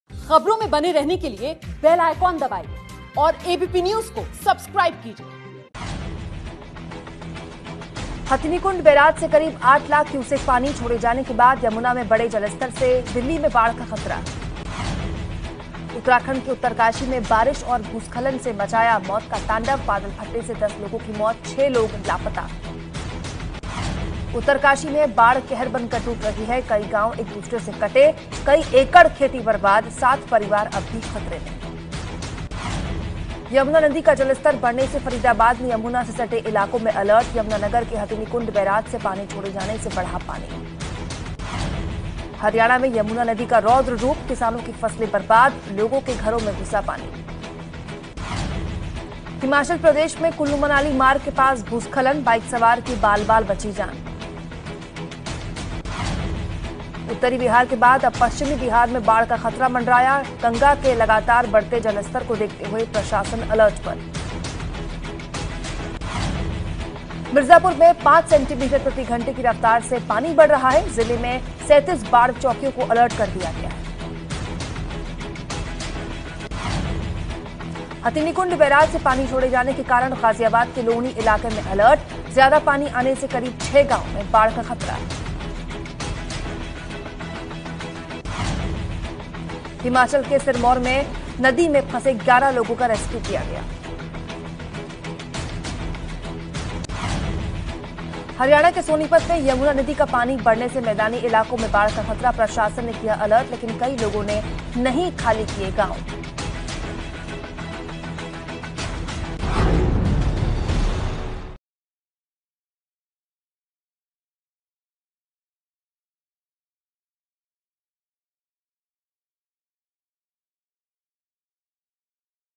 हथिनीकुंड से पानी छोड़े जाने के बाद यमुना का जलस्तर बढ़ा, दिल्ली में बाढ़ का खतरा | ABP News Hindi
हथिनीकुंड बैराज से करीब 8 लाख क्यूसेक पानी छोड़े जाने के बाद...यमुना में बढ़े जलस्तर से दिल्ली में बाढ़ का खतरा.
उत्तराखंड के उत्तरकाशी में बारिश और भूस्खलन ने मचाया मौत का तांडव, बादल फटने से 10 लोगों की मौत, 6 लोग लापता बताए जा रहे हैं. उत्तरकाशी में बाढ़ कहर बन कर टूट रही है, कई गांव एक दूसरे से कटे, कई एकड़ खेती बर्बाद हुई, कई परिवार अभी भी खतरे में.